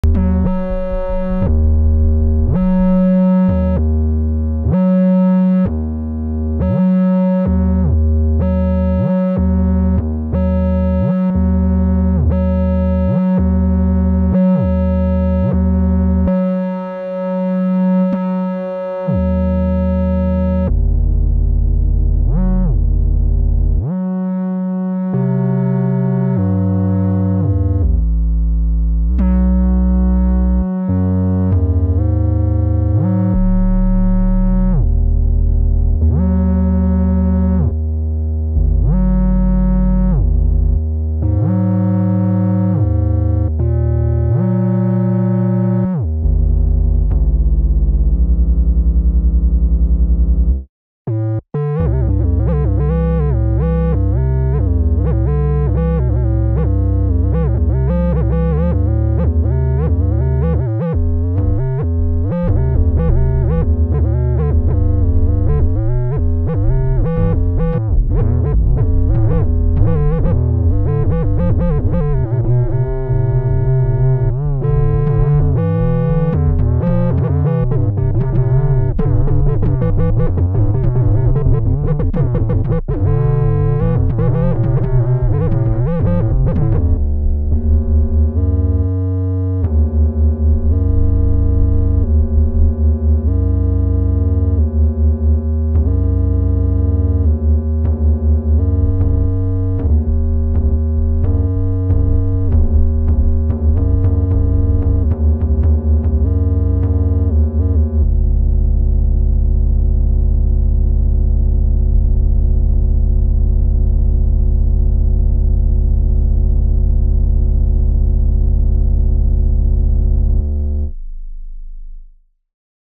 Going inside of a black hole ( theoretical not actual noises) by Brady Rowe
What going into a black hole would sound like in a space ship.